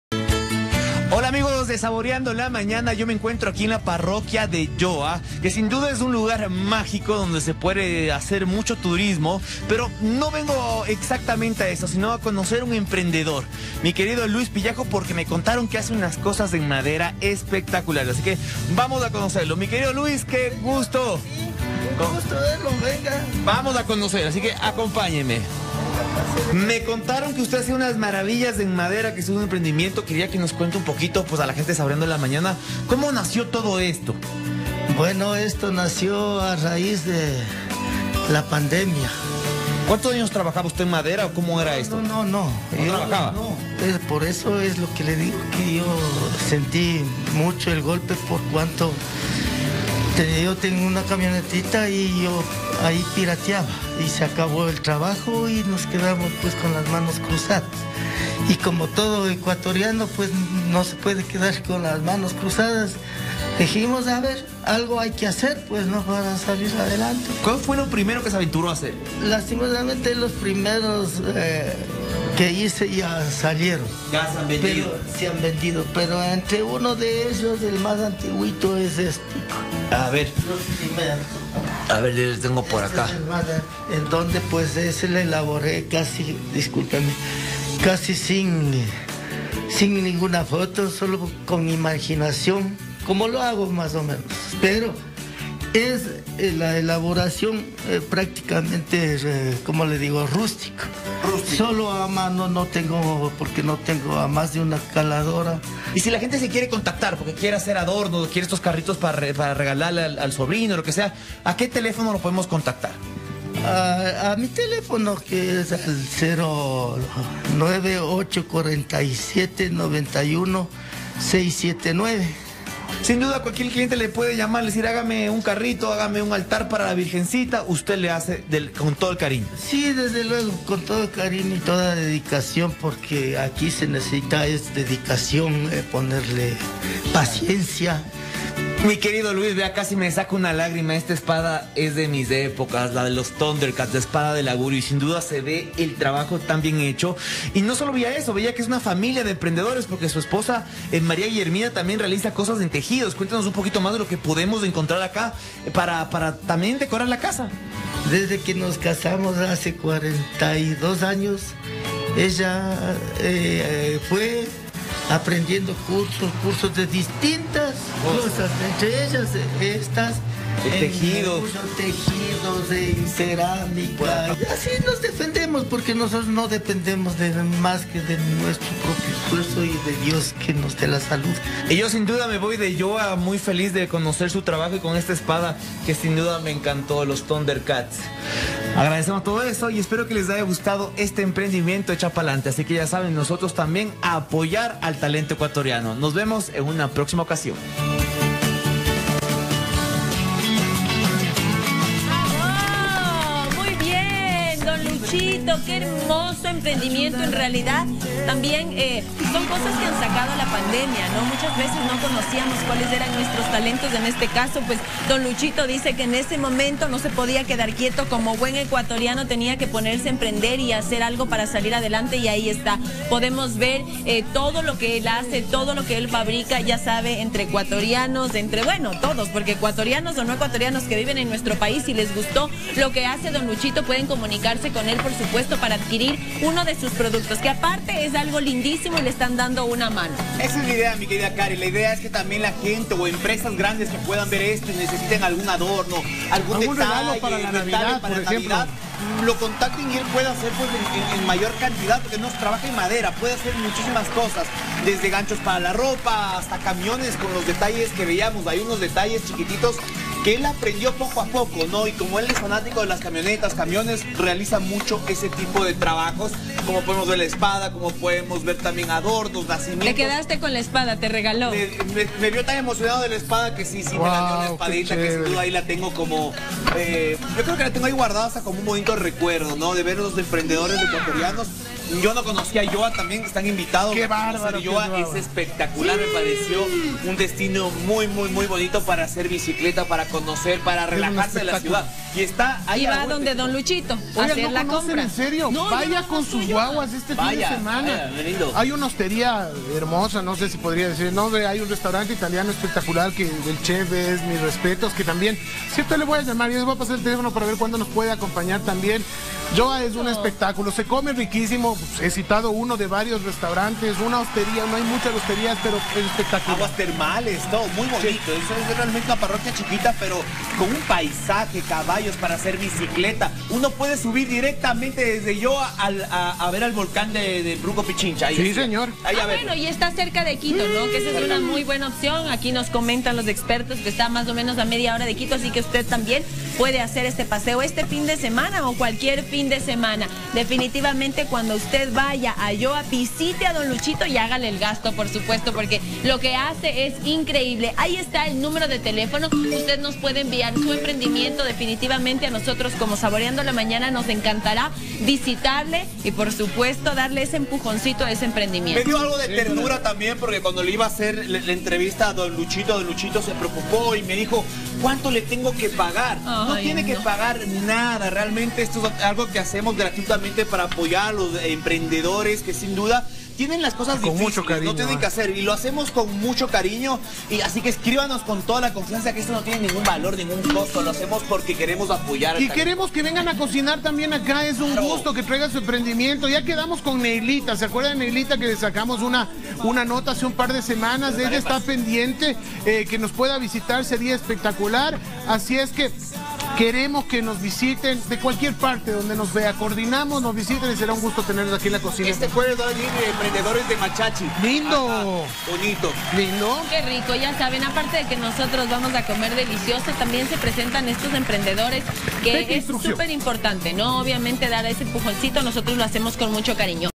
Un emprendedor que 'Echa Pa' Lante' en la Parroquia de Lloa
Luis Pillajo es un artesano que confecciona diferentes productos en la Parroquia de Lloa. Este emprendedor impulsó su idea en plena pandemia del Covid-19.